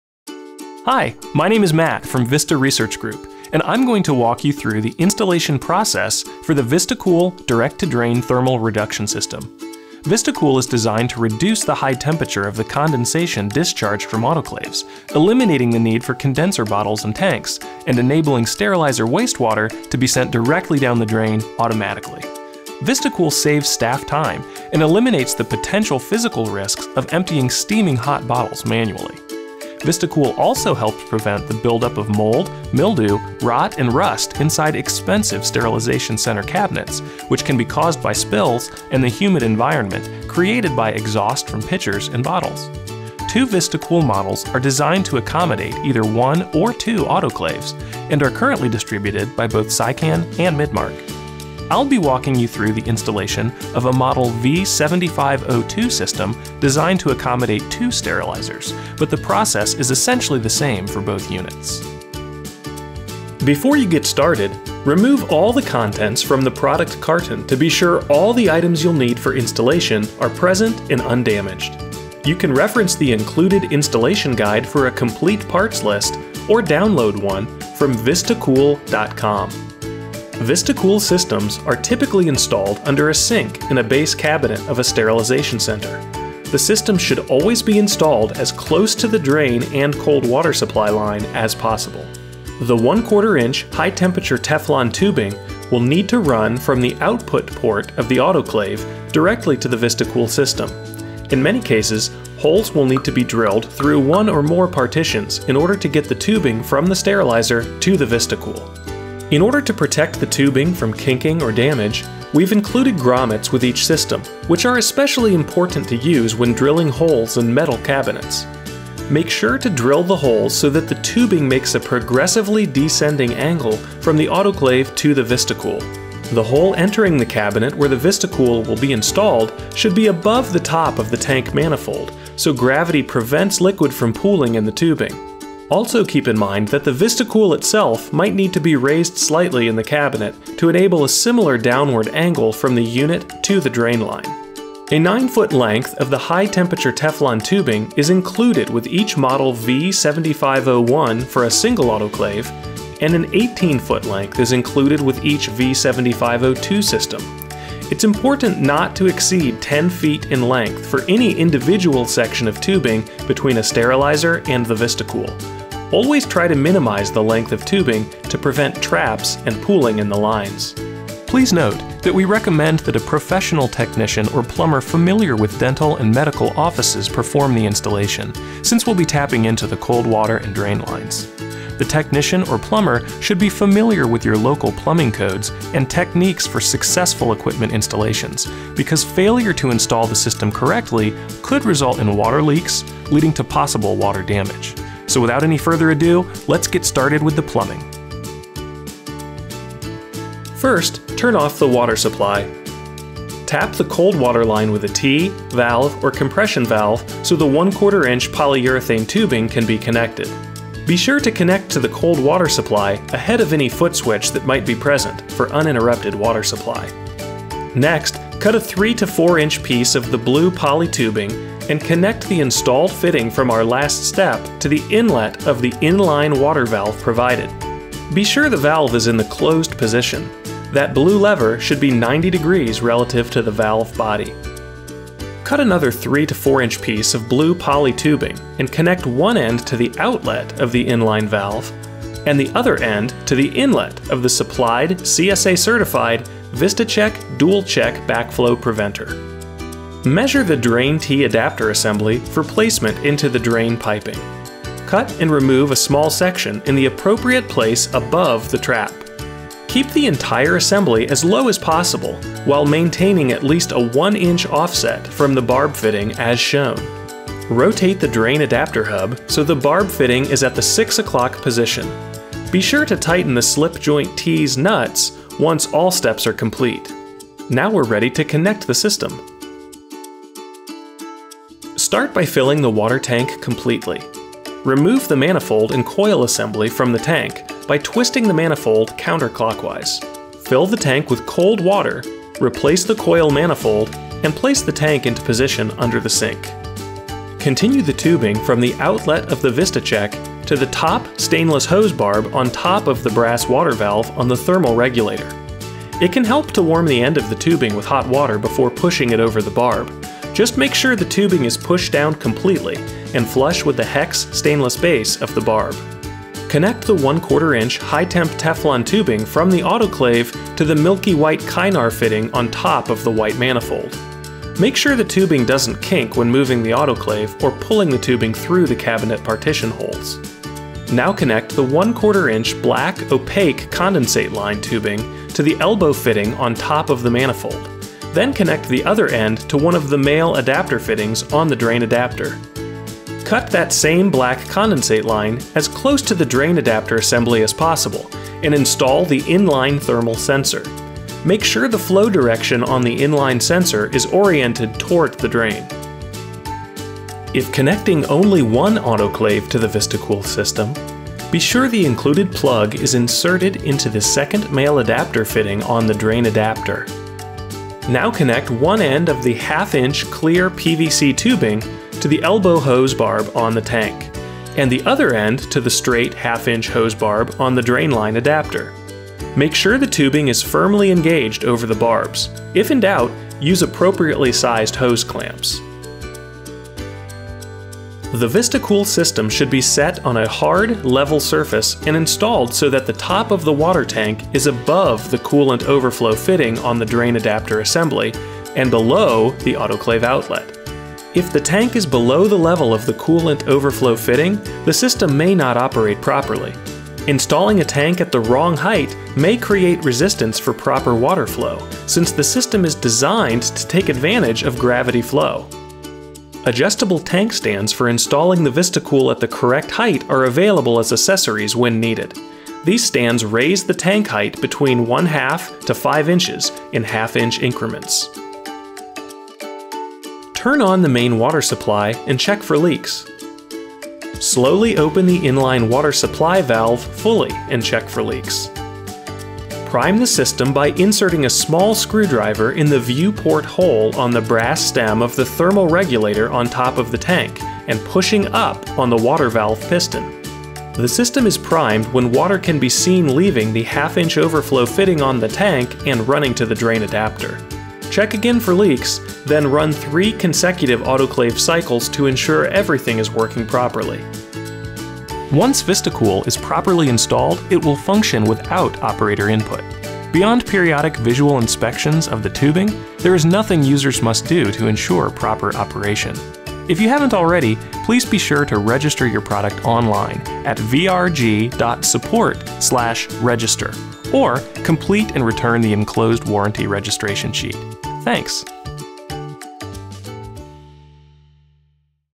VistaCool Installation Guide
We'll walk you through how to install your new VistaCool direct-to-drain thermal reduction system, which dramatically reduces the temperature of autoclave wastewater—eliminating condenser bottles and tanks!